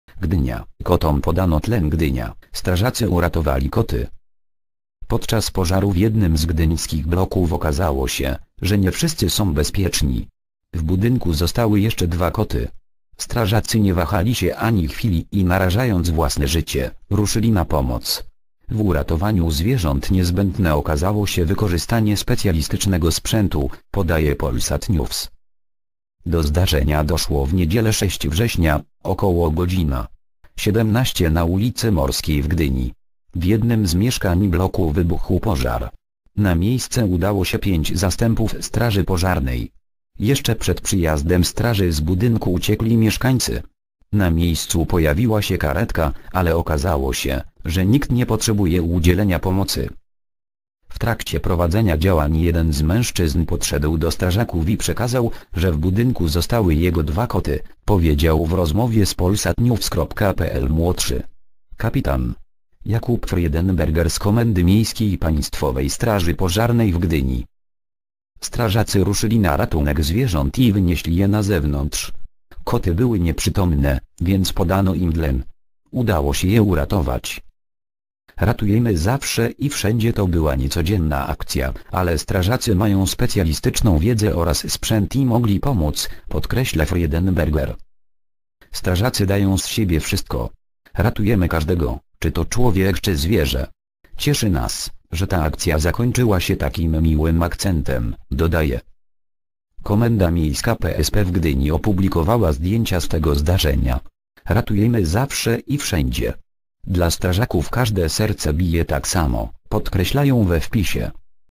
Gdynia: Kotom podano tlen.
Gdynia: Strażacy uratowali koty.

  (fot. NSW2 - zdjęcie ilustracyjne)